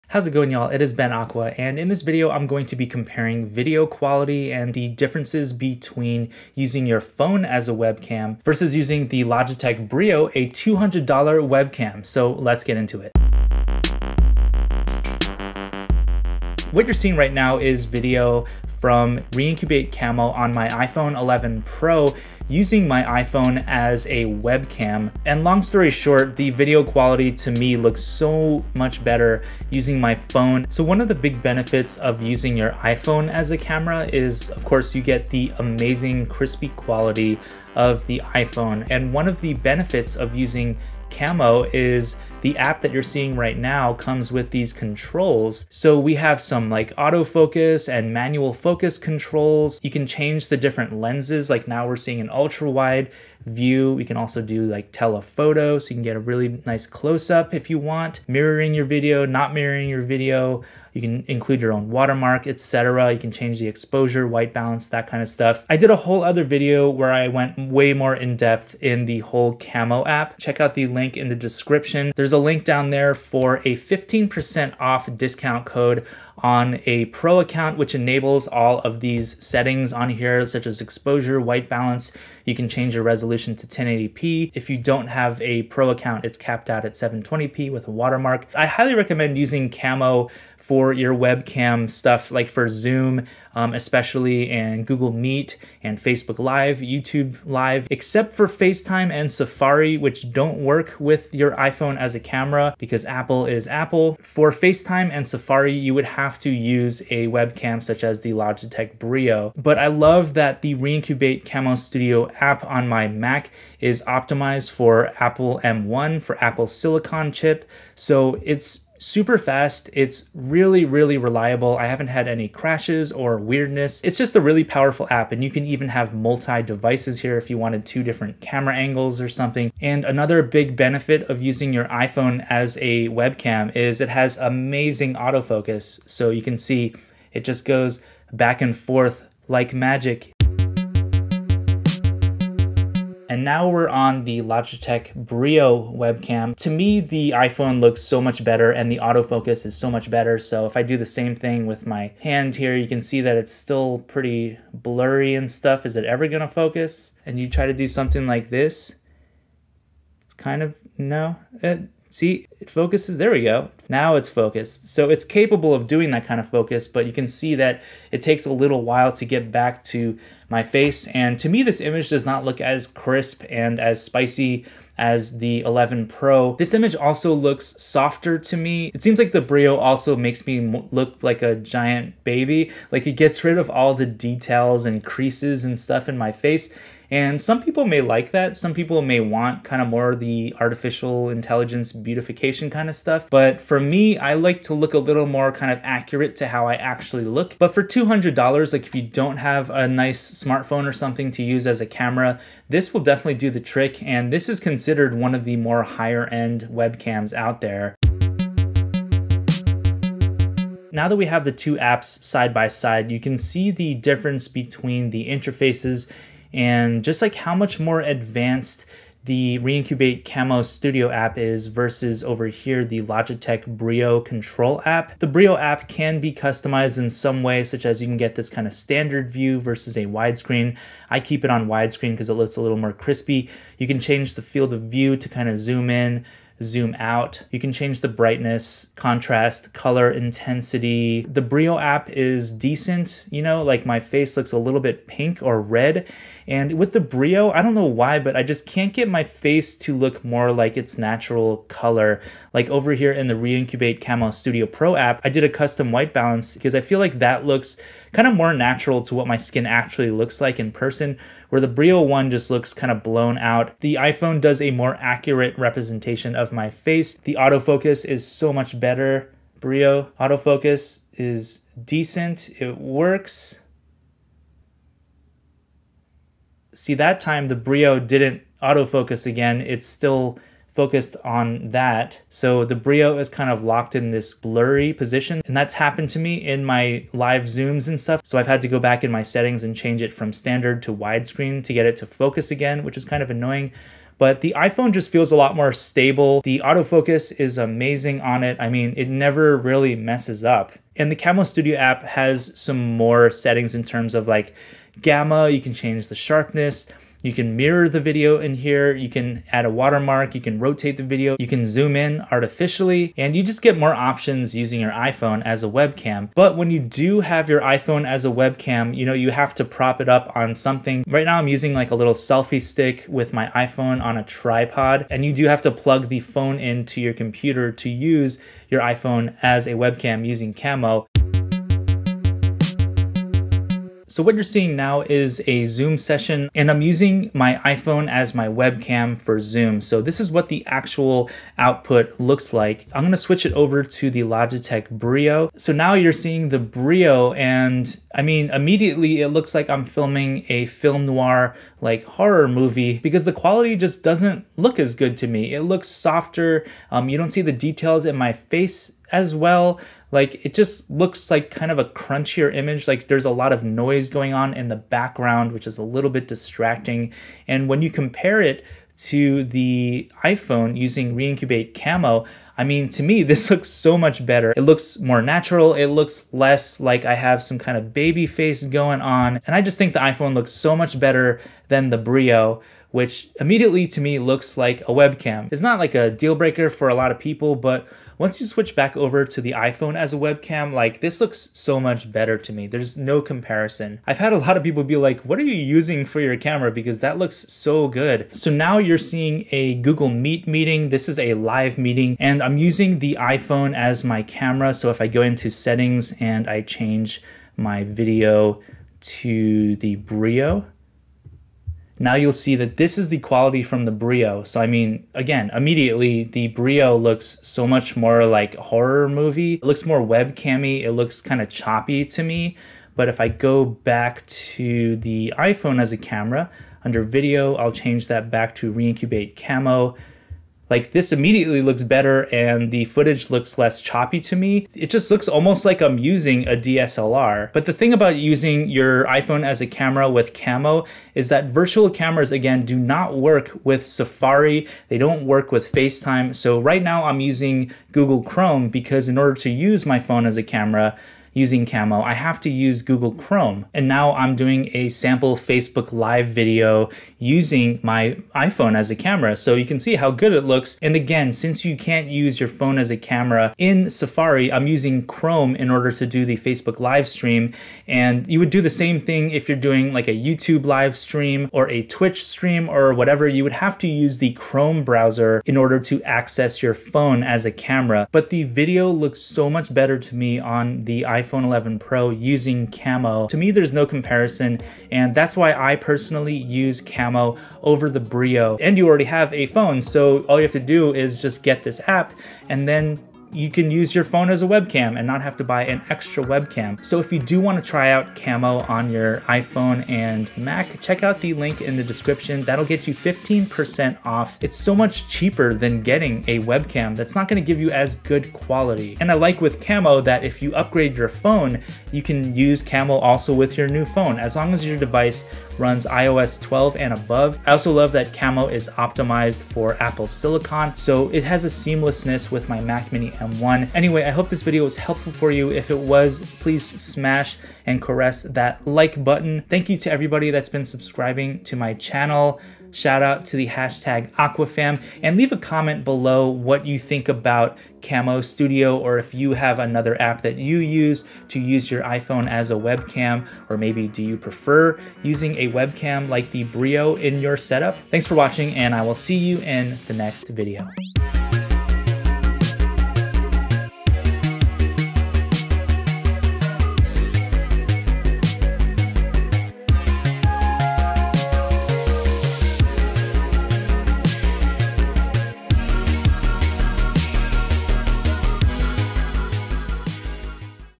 Logitech BRIO vs iPhone Camo app: BEST WEBCAM for Mac?
Logitech BRIO webcam
My selfie stick tripod
My desk lamp (BenQ e-Reading LED)
My microphone (RODE NT-USB Mini)

Your iPhone = the best webcam for Mac mini M1, MacBook Pro & Air M1! With an app called Camo (by Reincubate) and Camo Studio for Mac/PC, you can use your iPhone for live-streaming and WFH video calls in Zoom, Google Meet, Facebook Live, YouTube Live, Twitch, OBS, etc. The quality is AMAZING - better than Logitech BRIO and Streamcam. Camo also works for an easy and high-quality multi-camera setup! It has a portrait mode for blurry backgrounds, too. Plus, it's optimized for Apple Silicon/M1 chip! yasssss. In this video, I demonstrate using an iPhone 11 Pro w/ Camo vs the Logitech BRIO webcam. What's the best webcam for Mac M1?

Music is by me! Search "Ben Aqua" on Spotify & Apple Music ➜

0:00 iPhone as webcam?
0:39 iPhone 11 Pro x Camo app
2:20 Let’s try the BRIO
2:57 side-by-side image comparison
3:28 App comparison
5:31 Zoom, Meet, FB Live demo
7:58 Which is better?